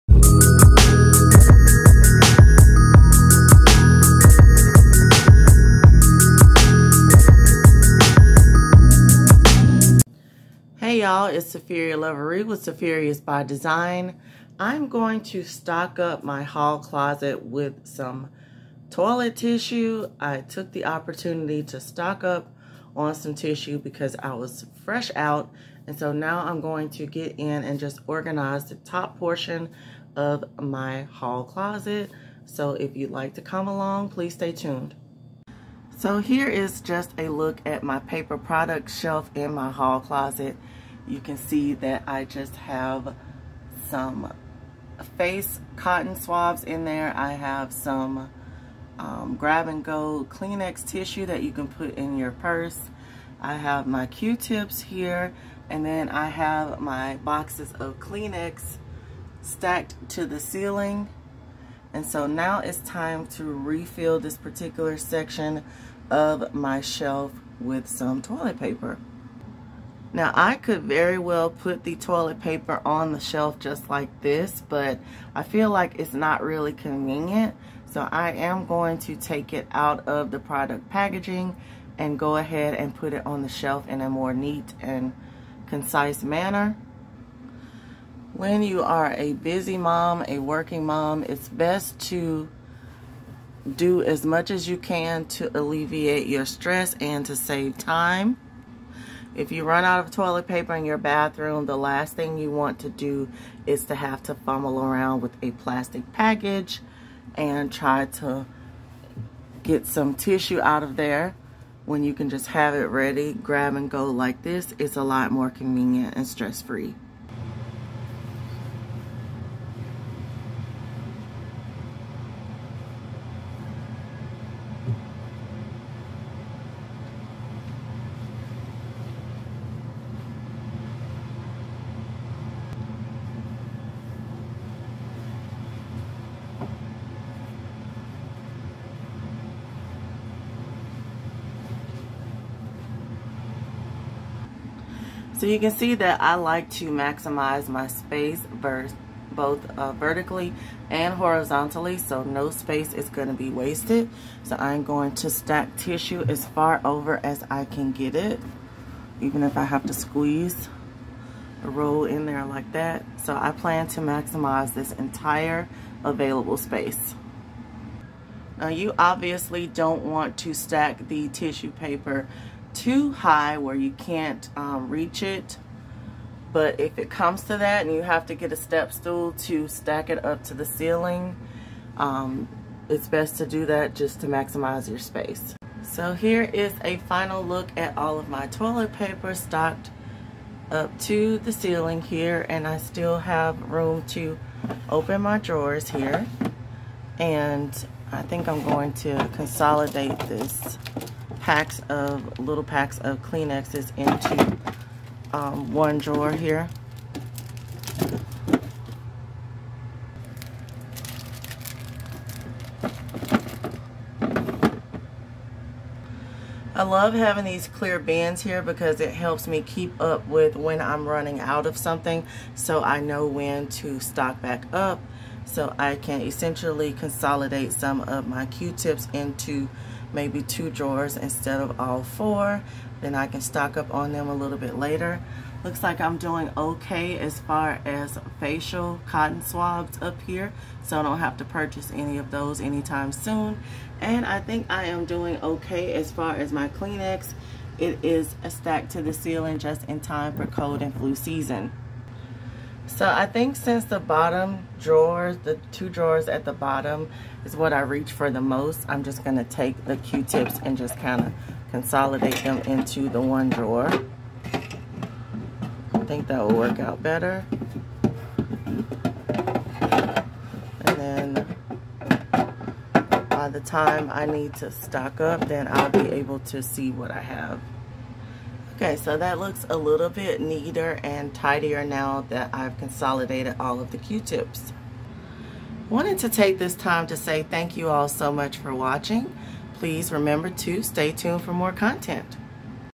Toilet Paper Organization
See how I organize all of my paper products on one shelf in my hall closet. All of my toilet tissue, kleenex, q-tips, and cotton rounds all fit neatly and perfectly on one shelf in my closet. In this video, you will see how I fit my stockpile of 45 rolls of tissue on one shelf by maximizing my vertical and horizontal space. Use all of the available space you have.